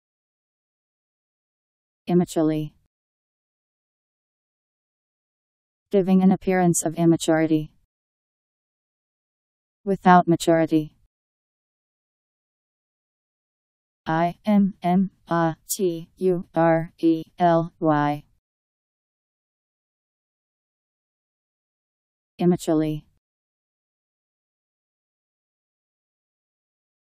Immaturely Meaning
Video shows what immaturely means. Giving an appearance of immaturity.. Without maturity..  Immaturely Meaning. How to pronounce, definition audio dictionary. How to say immaturely. Powered by MaryTTS, Wiktionary